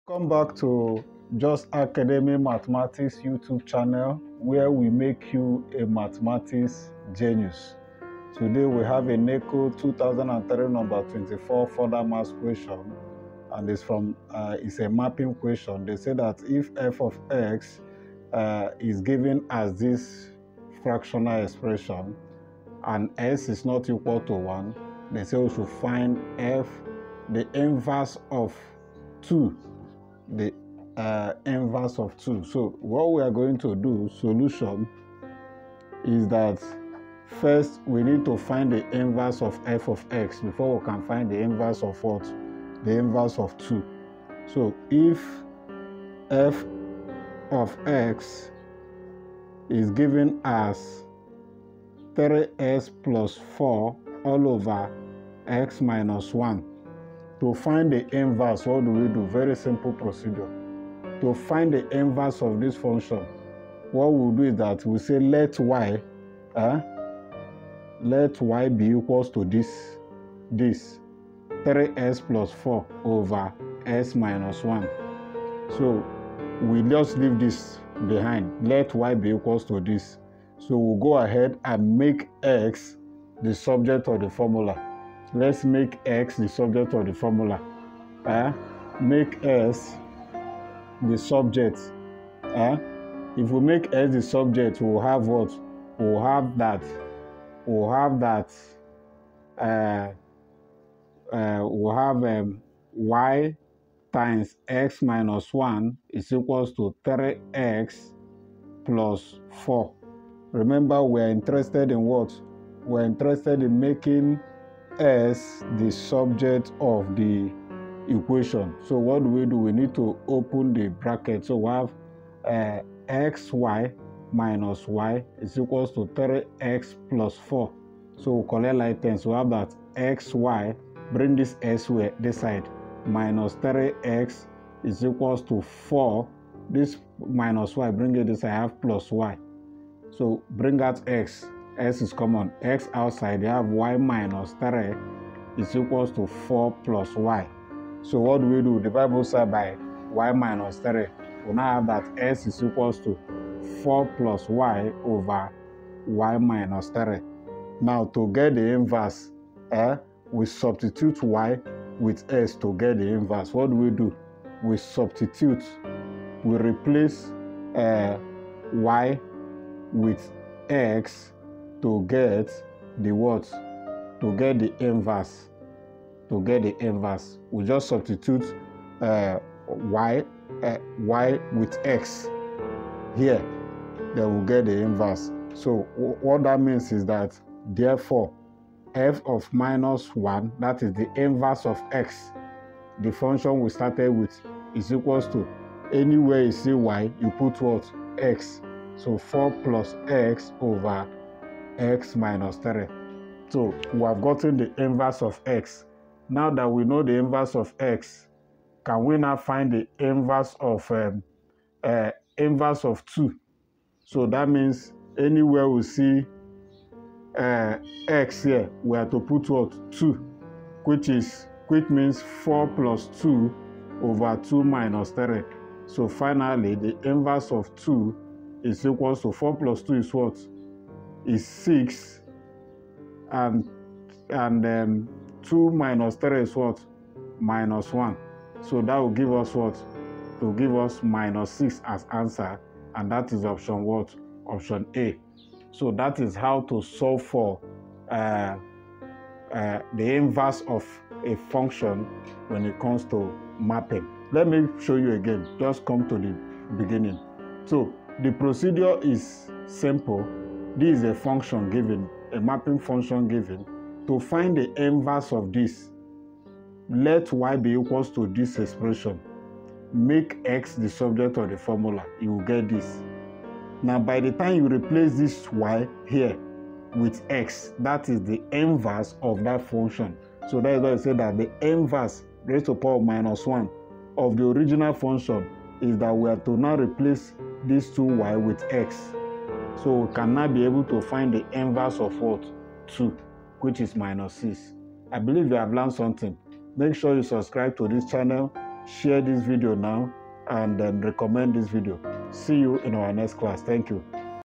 Inverse of a Function - By A. C. Okocha
In this video, you will learn how to calculate the inverse of a given algebraic expression.